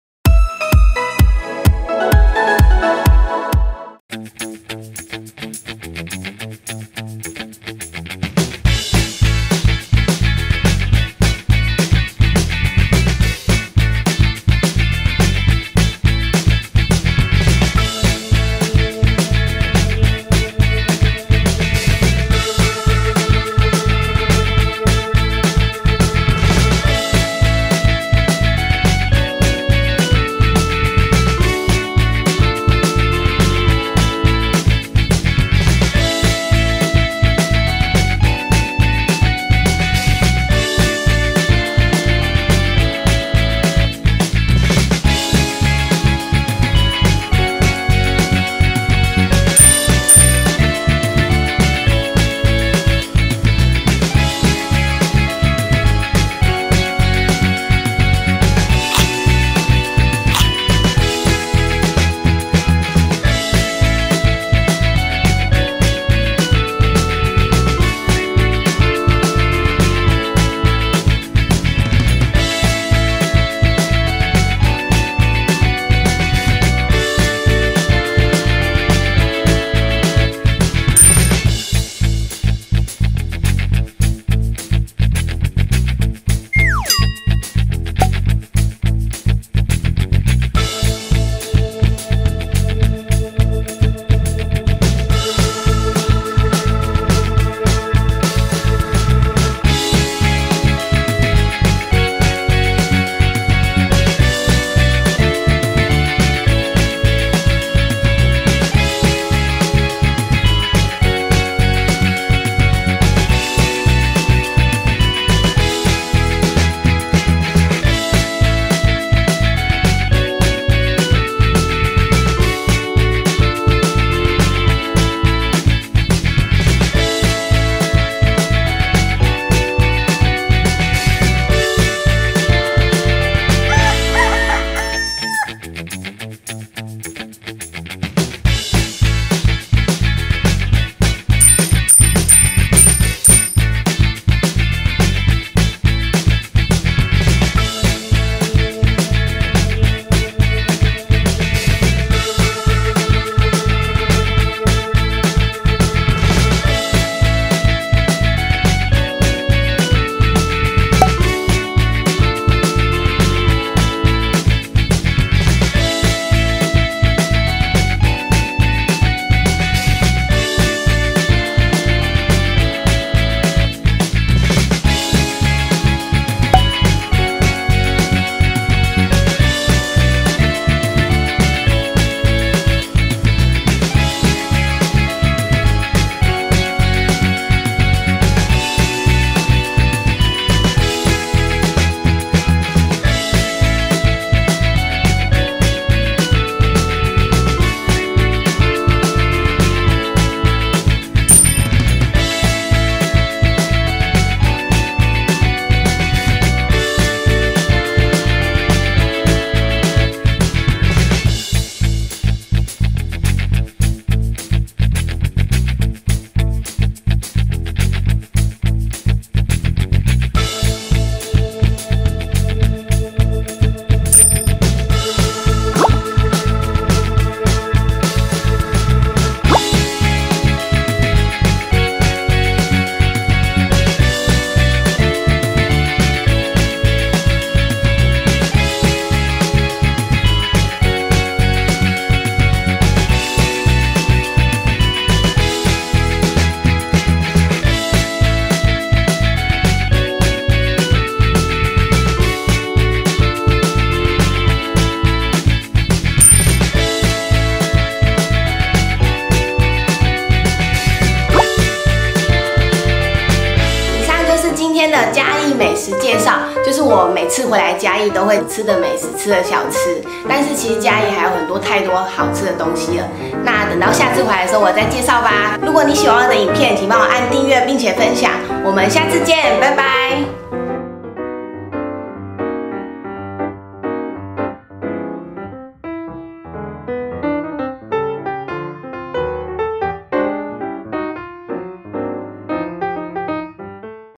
嘉義人站出來!我從小吃到大的嘉義在地美食!---李欣FOOD#3
繼上次的嘉義網美必拍景點介紹～
這次要來告訴你們好吃的美食囉～
❤開啟鈴鐺就可以收到新影片通知哦～
⇩歡迎在下方留言告訴我你的意見及想法
➤東門火婆煎粿
營業時間: 06:30-11:30
地址: 嘉義市東區公明路188號

➤二丫頭麻辣涼麵
營業時間: 08:00-21:00
地址: 嘉義市東區民族路170號

➤和平火雞肉飯
營業時間: 07:00-20:30
地址: 嘉義市東區和平路107號

➤台灣人的冰
營業時間: 00:40-20:30
地址: 嘉義市蘭井街249號

➤國華街無名米糕
營業時間: 16:00-19:00
地址: 嘉義市國華街96號

➤文化路酪梨牛奶
營業時間: 不定
地址: 嘉義市文化路134號